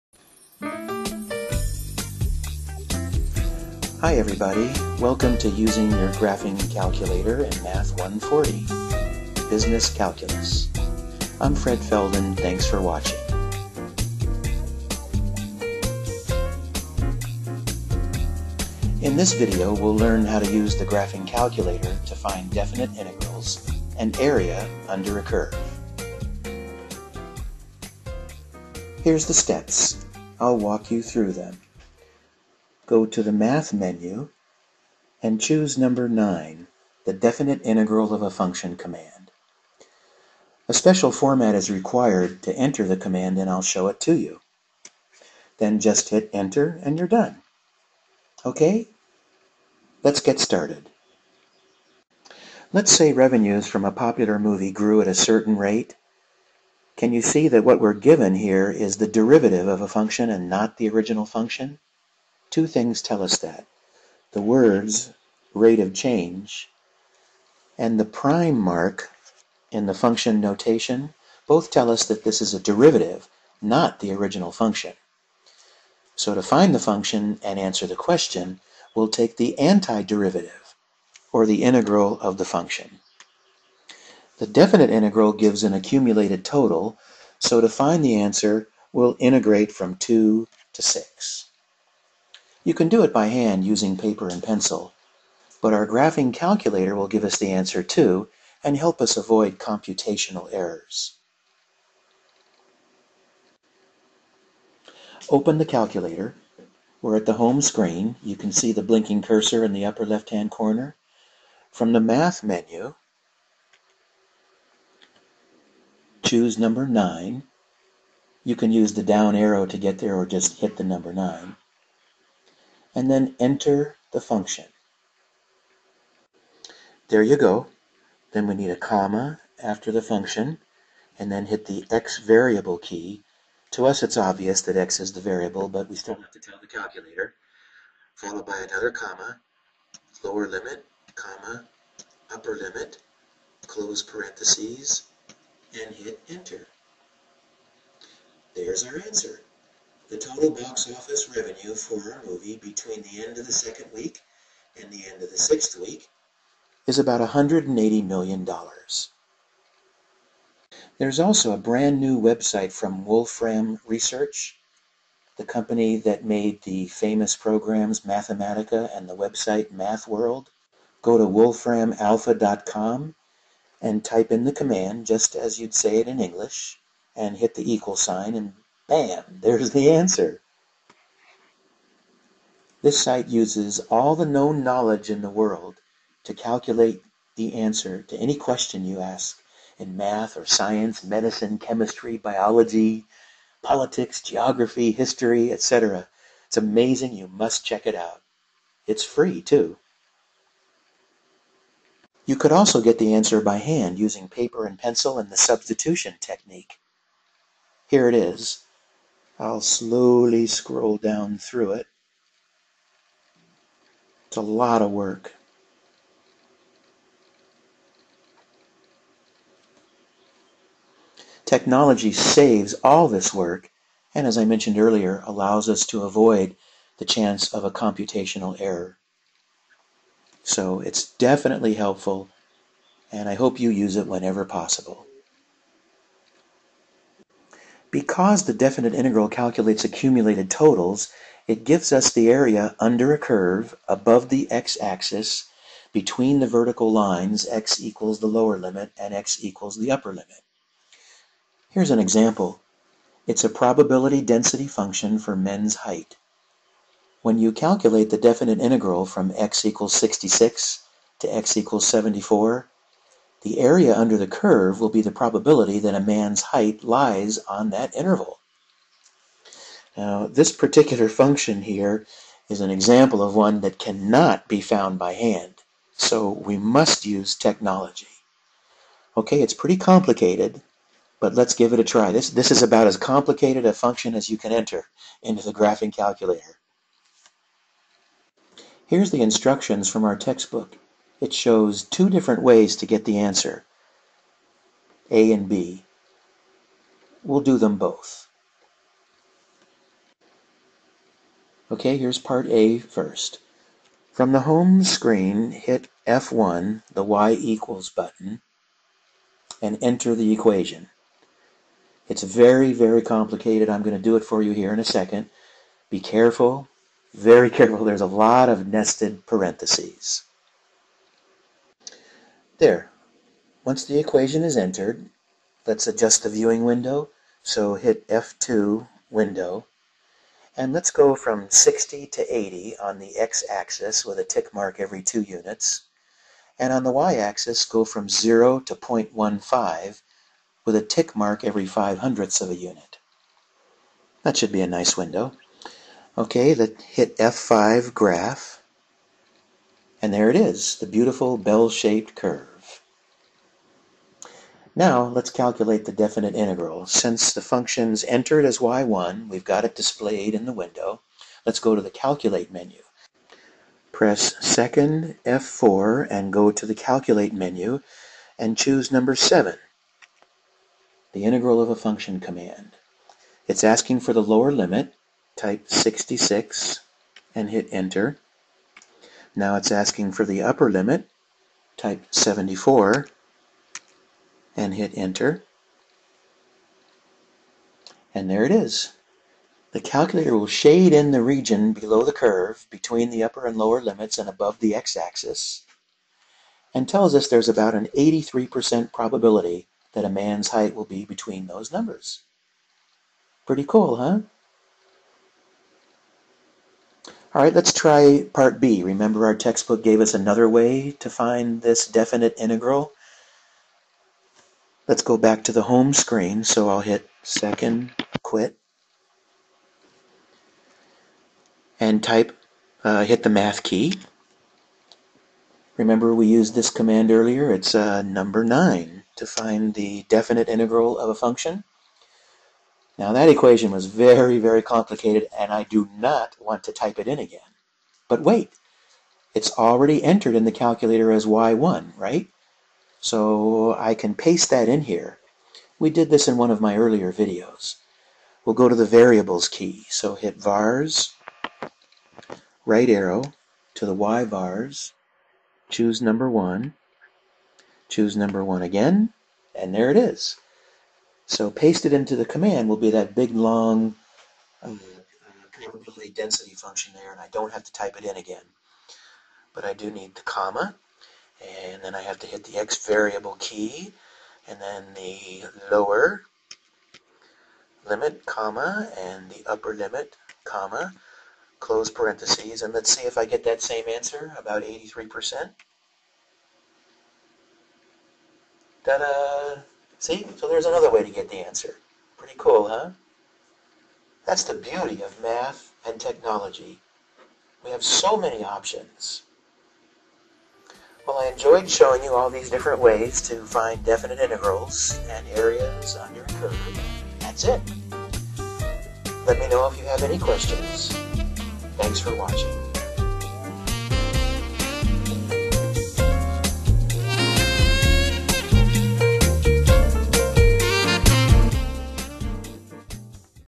TI Video: Definite Integrals
Using technology to calculate definite integrals and area under a curve.